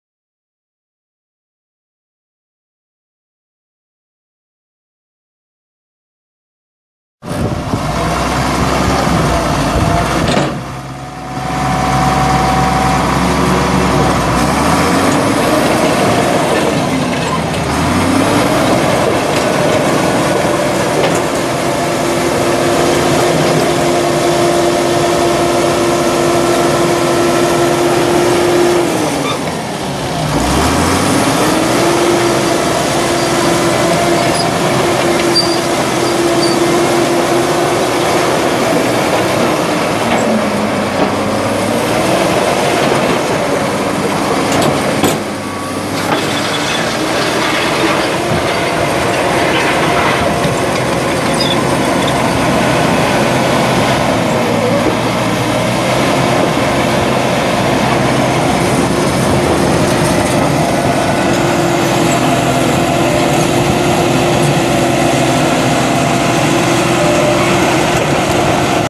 1975 International 175C track loader for sale | no-reserve Internet auction September 29, 2016
1975 International 175C track loader
9,566 hours on meter
International DT466B 7.63L six cylinder turbo diesel engine
100 HP
Powershift transmission
4F-4R gears
ROPS
Joystick bucket controls
86"W multi-purpose bucket
1.5 cu. yd. capacity
16"W triple grouser track pads
31,670 lbs. base operating weight
Serial 4290001U002621

Item location address: This item is located at 18255 Marten Road, Wamego, KS